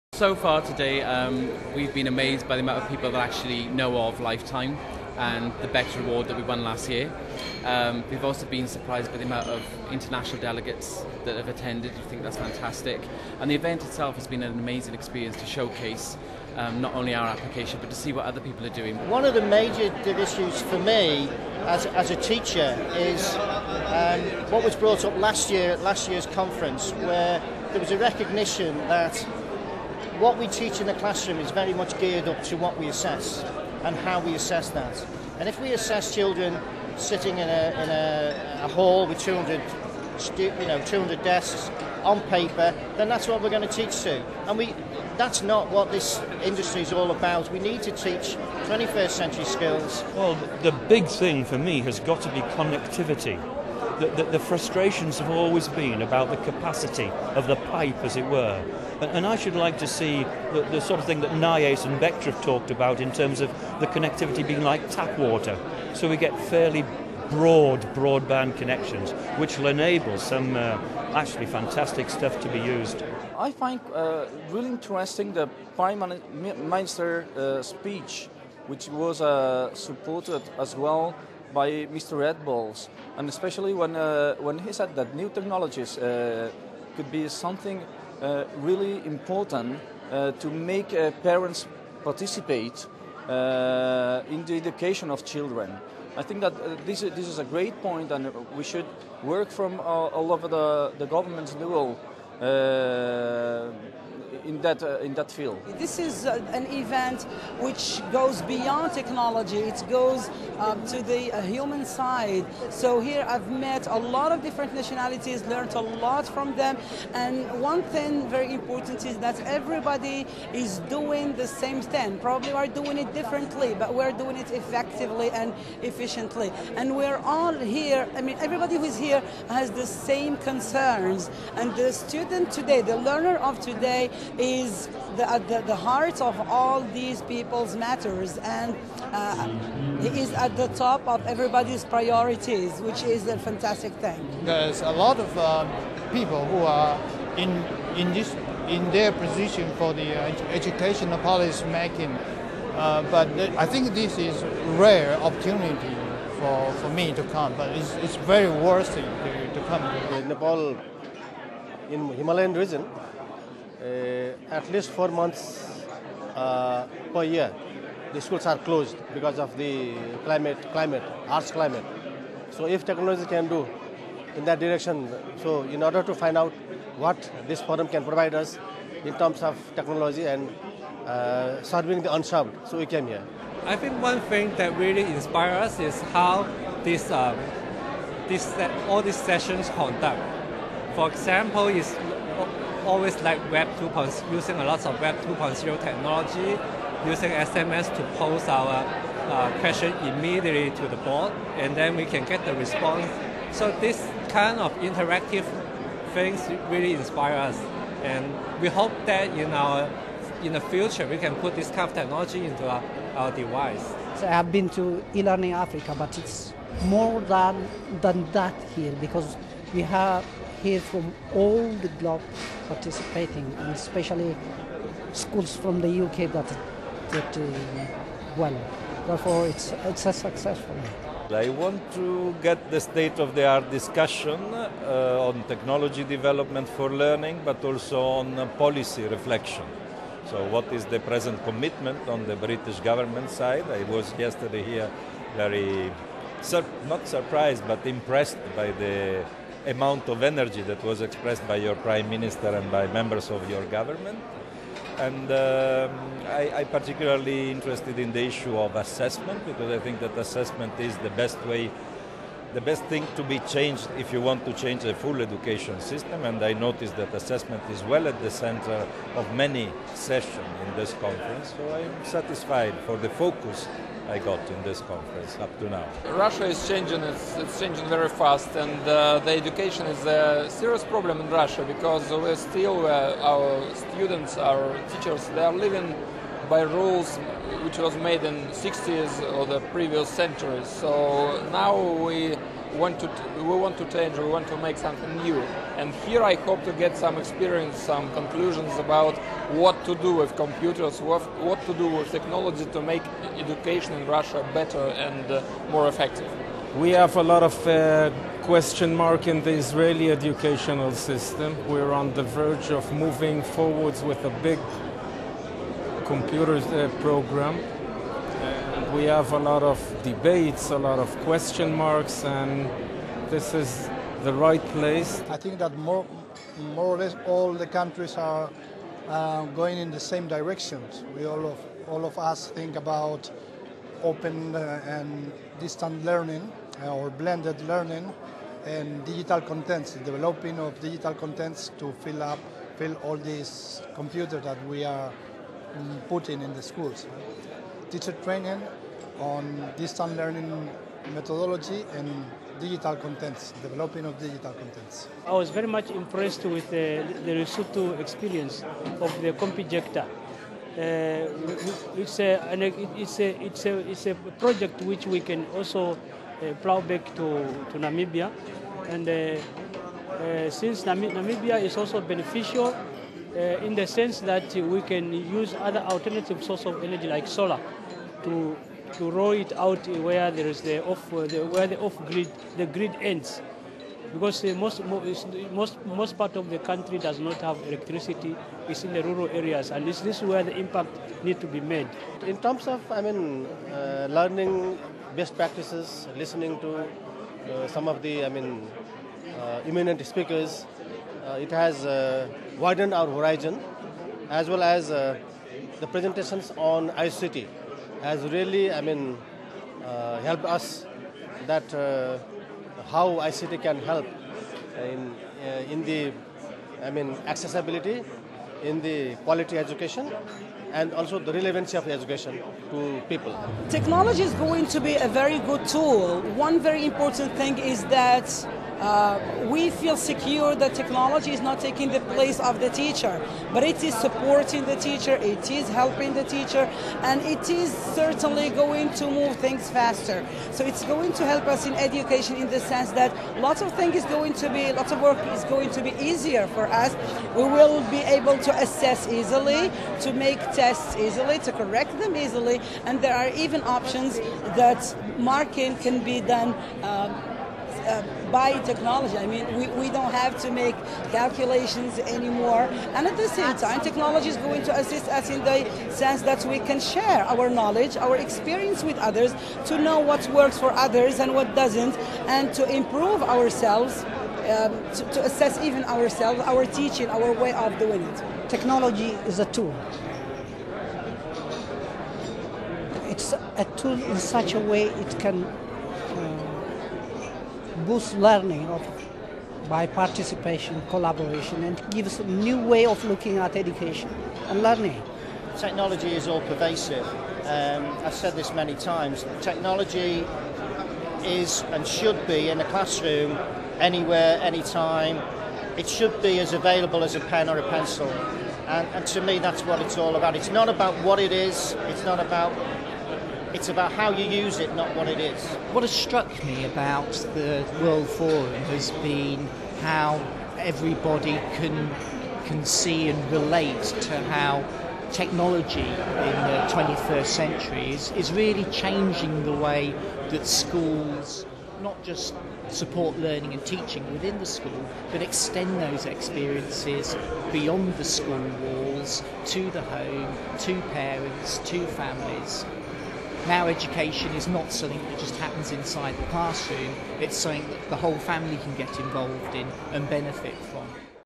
Delegate interviews at LATWF 2010
International ministers, education experts and industry professionals discuss how the Learning and Technology World Forum is leading the future of educational advances through technology.